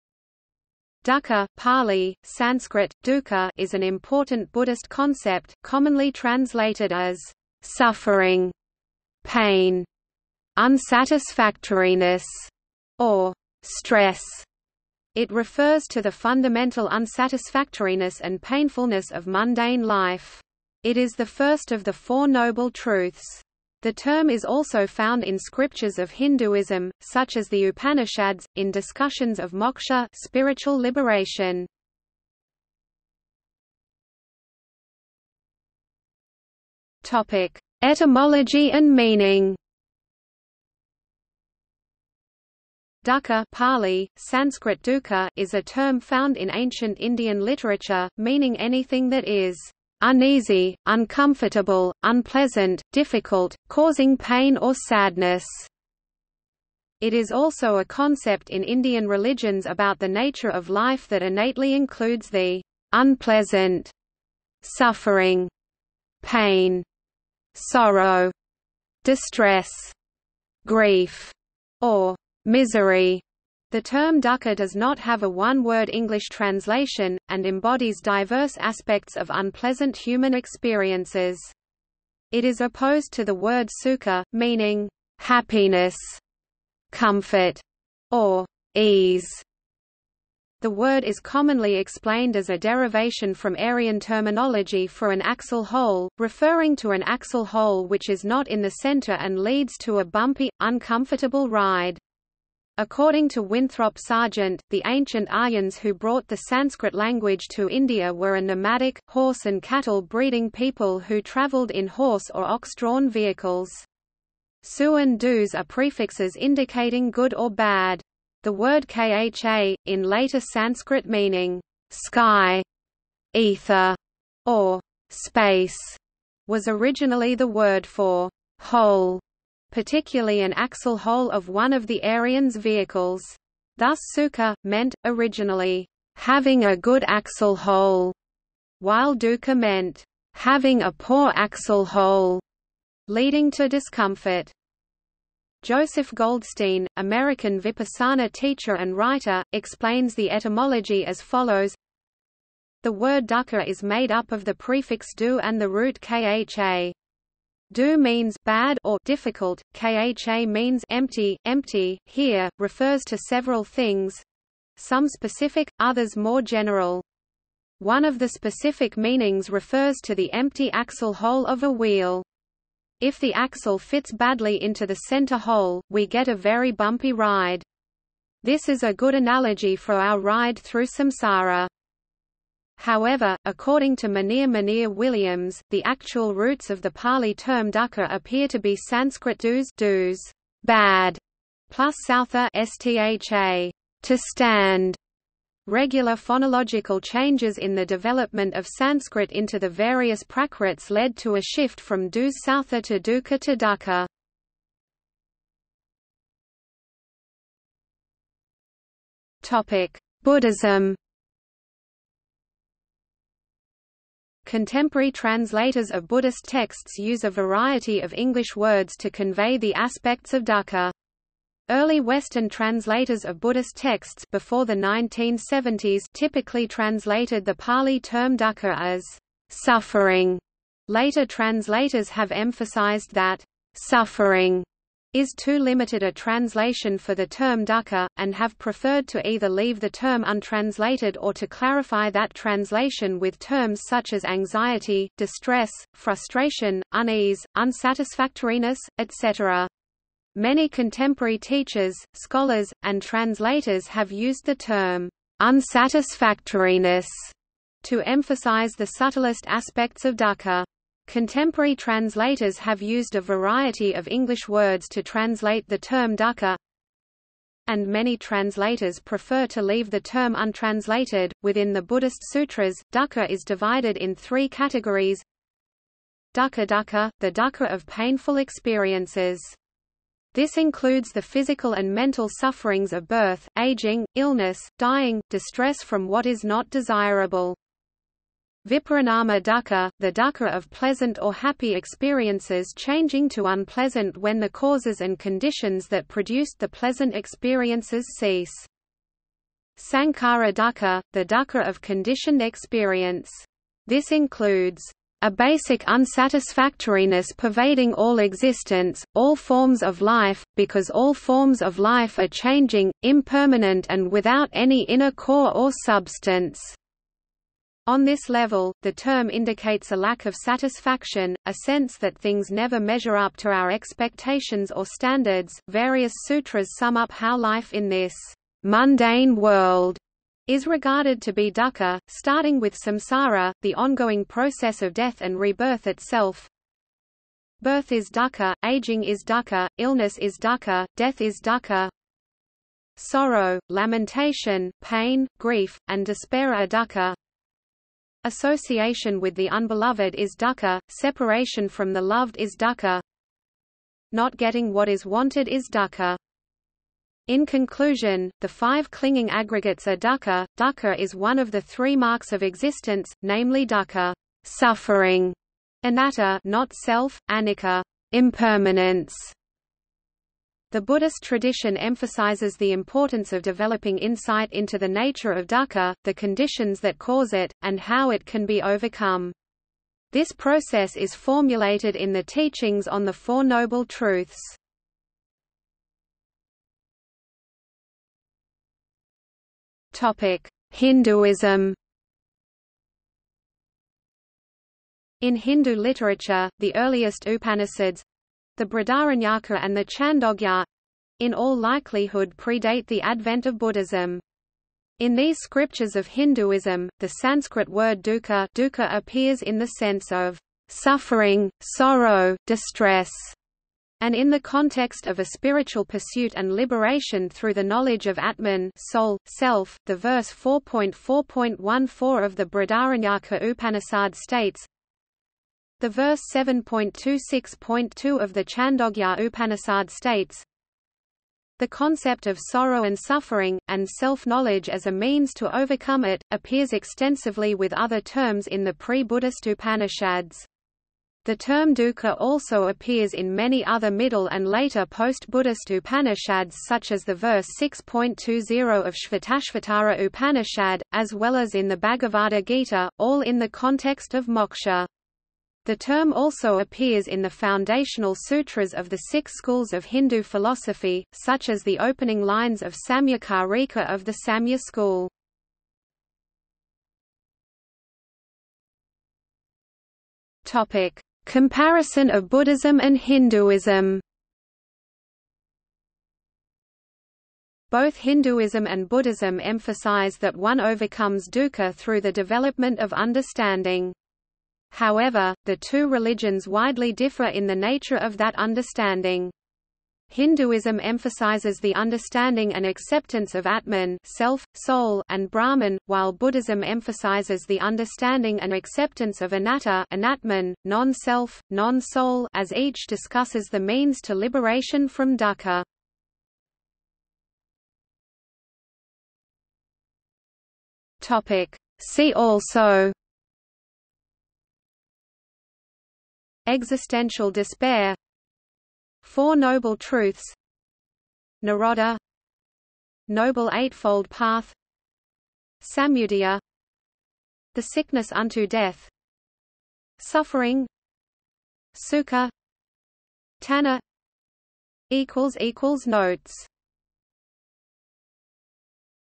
Dukkha | Wikipedia audio article
00:00:33 1 Etymology and meaning
00:03:25 2 Buddhism
00:06:26 3 Hinduism
00:07:47 4 Comparison of Buddhism and Hinduism
00:08:27 5 See also

- Socrates

SUMMARY
Dukkha (; Pāli; Sanskrit: duḥkha) is an important Buddhist concept, commonly translated as "suffering", "pain", "unsatisfactoriness" or "stress". It refers to the fundamental unsatisfactoriness and painfulness of mundane life. It is the first of the Four Noble Truths. The term is also found in scriptures of Hinduism, such as the Upanishads, in discussions of moksha (spiritual liberation).